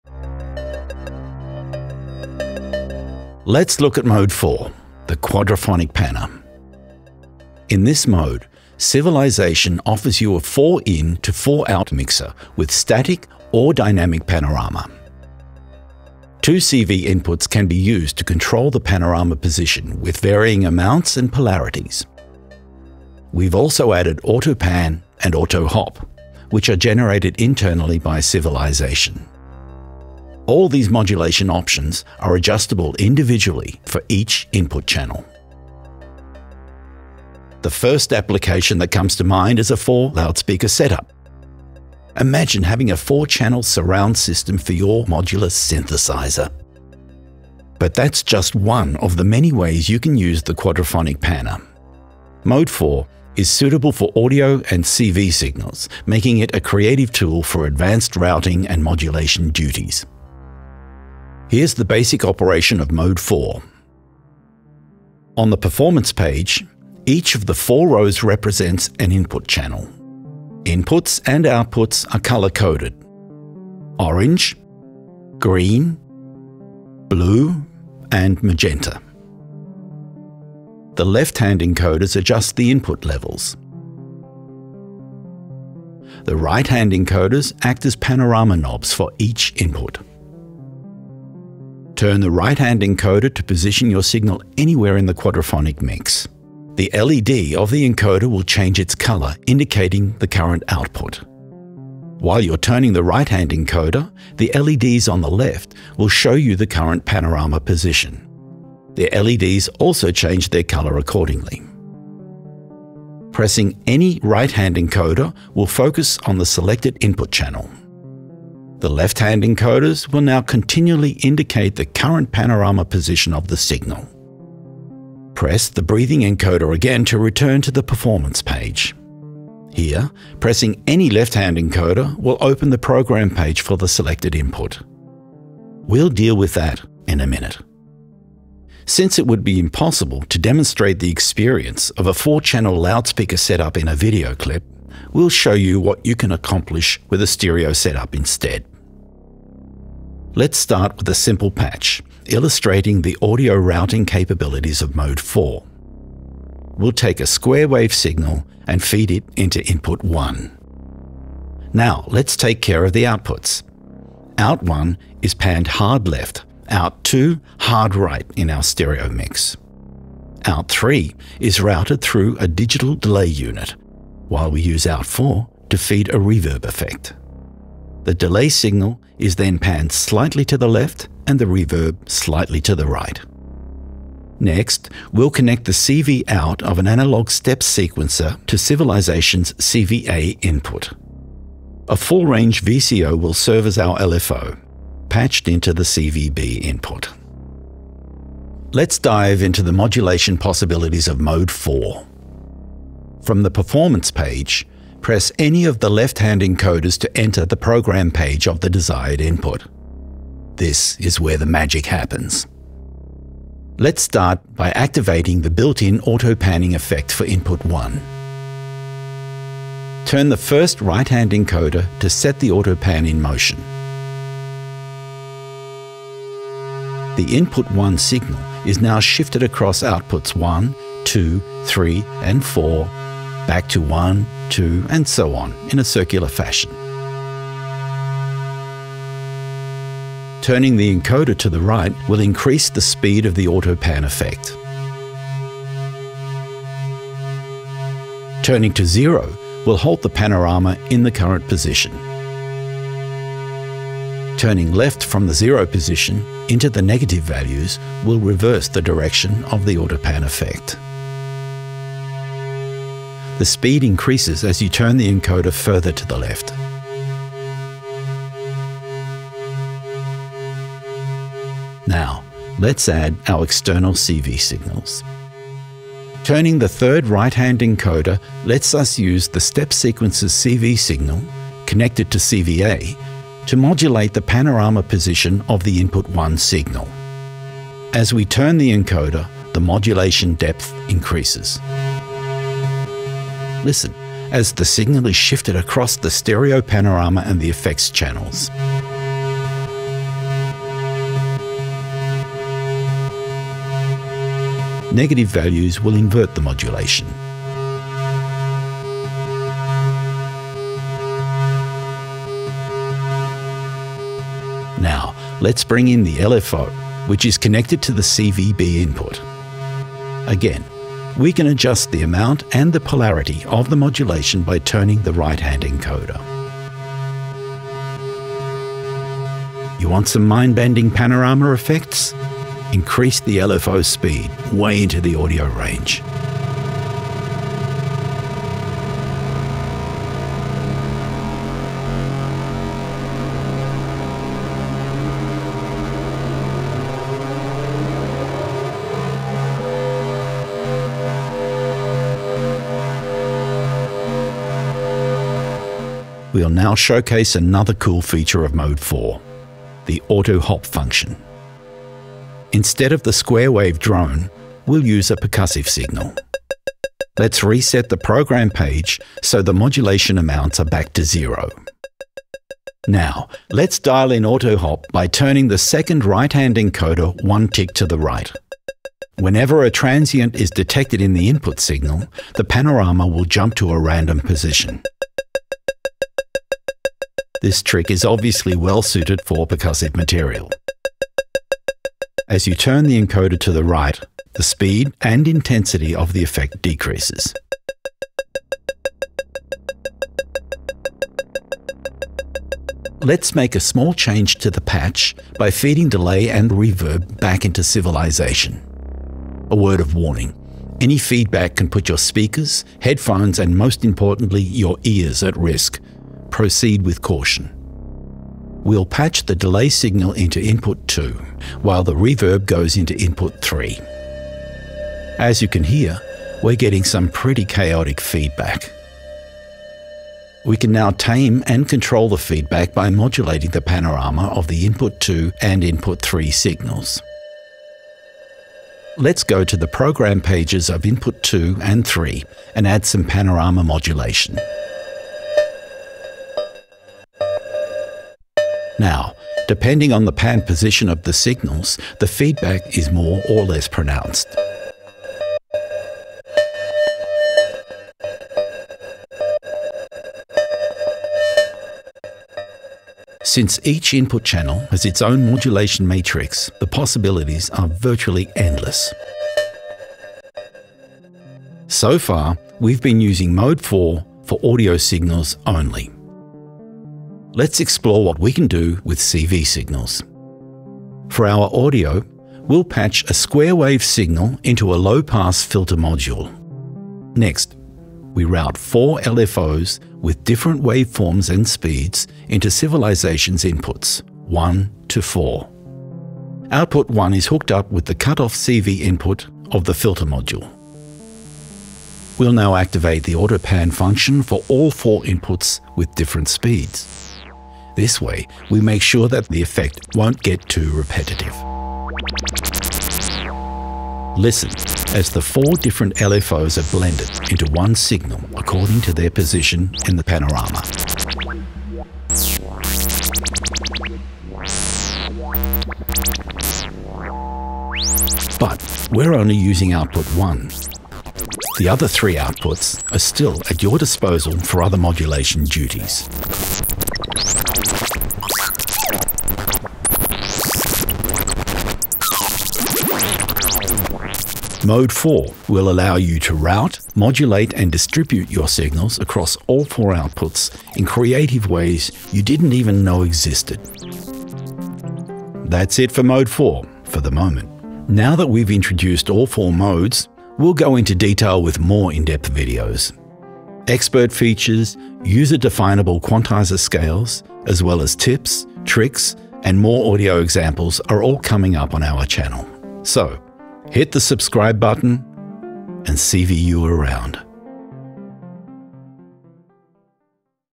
u-he CVilization Mode IV: Quadraphonic Panner
Mode IV offers a 4-input to 4-output mixer with dynamic panning i.e. crossfading. Ideal for distributing audio between 4 loudspeakers, Mode IV is also great for other tasks. Signals at the 2 CV inputs can modulate positions with varying amounts and polarities per channel. Mode IV also features “Auto-Pan” and “Auto-Hop”, so you don’t even need an external modulation source to move signals around. Mode IV can process Audio or CV signals, making it a highly creative and versatile tool for any Eurorack system.

00:00 - Intro
00:07 - Mode IV Key Features
01:05 - Performance Page / Input Channels
01:20 - Adjusting Input Levels & Panorama
01:31 - Basic Quadraphonic Panning
01:53 - Focus View
02:04 - Program Pages
02:18 - Stereophonic Patch
02:36 - Setting up Audio Signals
03:04 - Setting up CV-A & CV-B Inputs
03:23 - Program Pages
03:39 - Auto-Pan
04:36 - Pan Modulation via External CV (CV-A)
05:04 - Inverting Modulation
05:14 - Pan Modulation via External CV (CV-B)
05:29 - Audio-Range Pan Modulation
05:56 - Auto-Hop
06:42 - Audio Feedback
07:18 - Program Pages
07:51 - Mode IV for Control Voltages
08:33 - Blending 4 CV Sources into One
09:15 - Closing Remarks